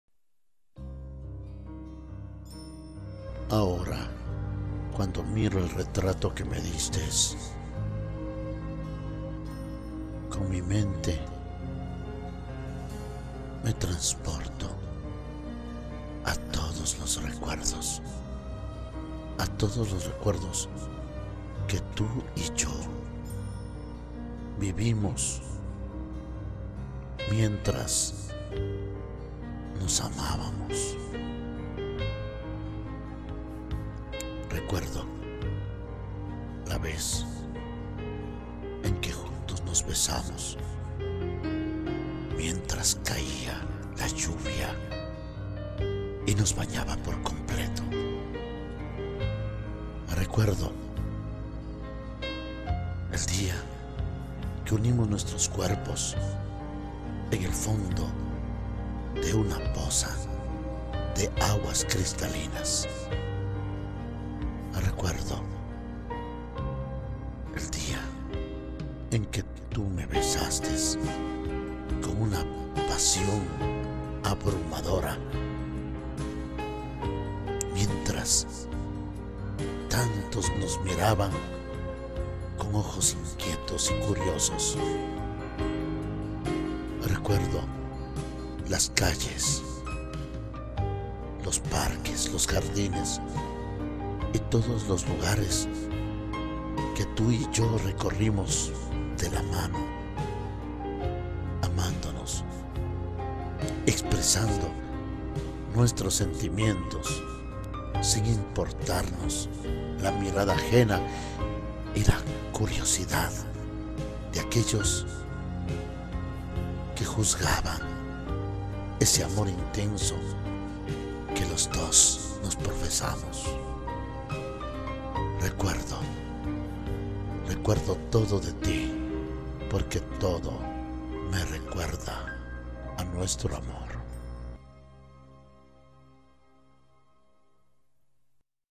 GRANDES RECUERDOS.- Rubén Patricio Aguirre
Rubén Patricio Aguirre, interpretando una de sus composiciones poéticas, lenguaje sencillo pero de profundo sentimiento.